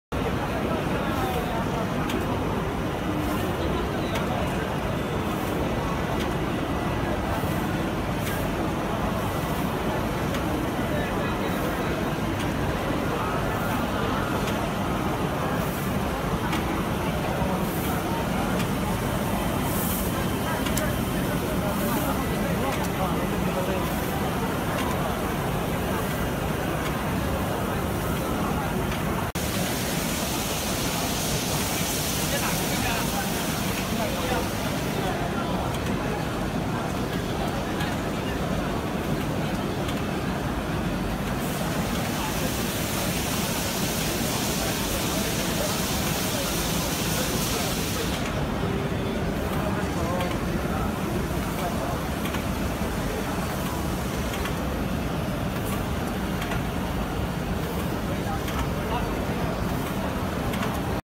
Fedar TC1948 Dye Sublimation Printer Textile Printing Machine
Fedar TC1948 Dye Sublimation Printer Textile Printing Machine with 8 Epson Original I3200-A1 heads, Max Printing Speed: 320Sqm/hr.